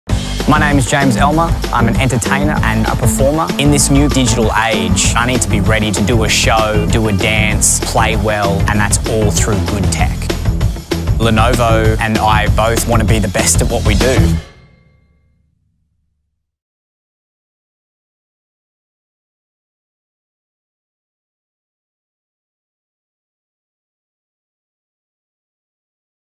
James Elmer x Creator Series 1
Dancing, singing and streaming - it's all part of James Elmer's digital repertoire. With Lenovo tech he can do it all.